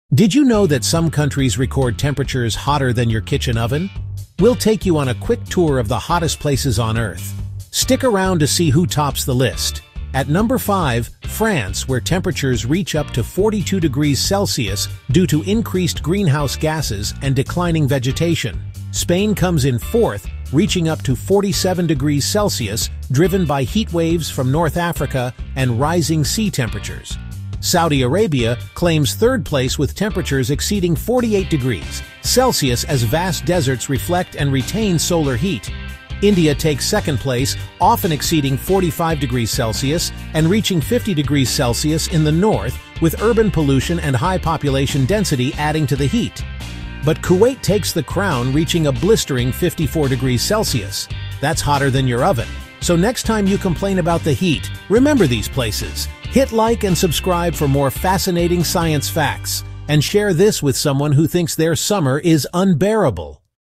Top 5 Hottest Countries in the World Right Now – You Won’t Believe Who’s Number One!
This short video ranks the top five hottest countries on Earth right now—from the least to the most extreme.
Why are temperatures soaring in places like France, Spain, Saudi Arabia, India, and Kuwait?
We break down the scientific reasons behind these heatwaves, including climate change, desert geography, and rising global temperatures.
Watch till the end for surprising facts and stay informed in under a minute.

ExtremeHeat